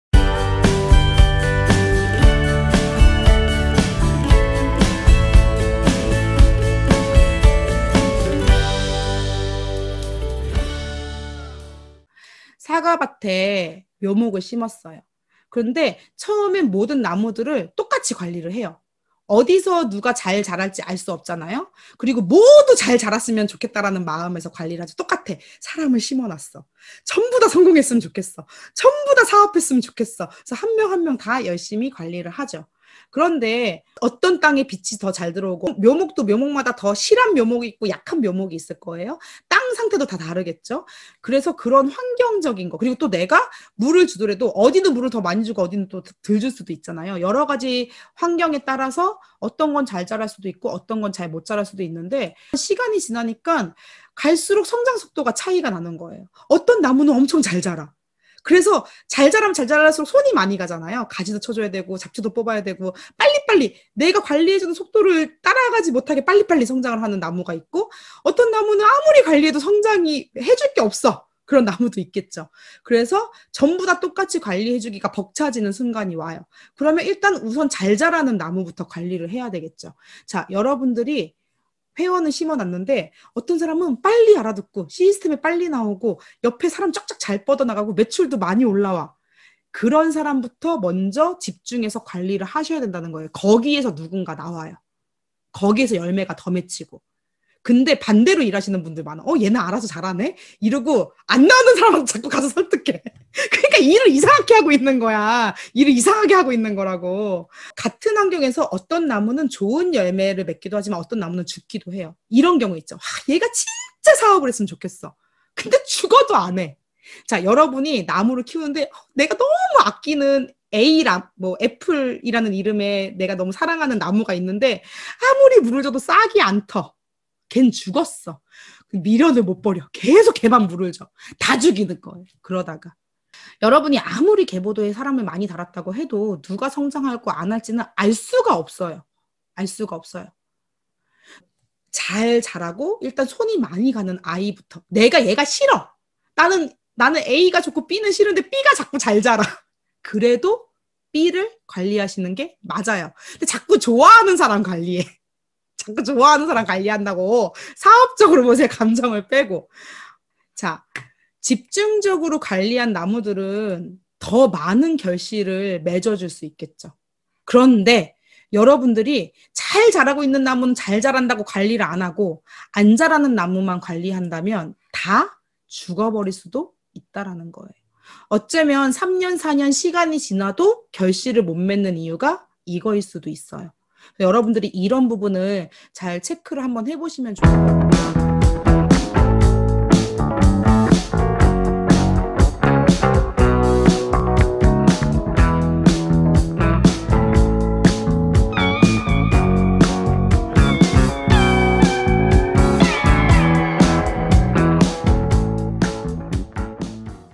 긍정의 원더우먼 | 김은혜 | 후속관리 노하우 선택과 집중
여러분 친구 만들려고 애터미 하는거 아니시죠?
애터미는 빅 비지니스입니다
선택과 집중을 잘 해야 사업이 잘 될겁니다~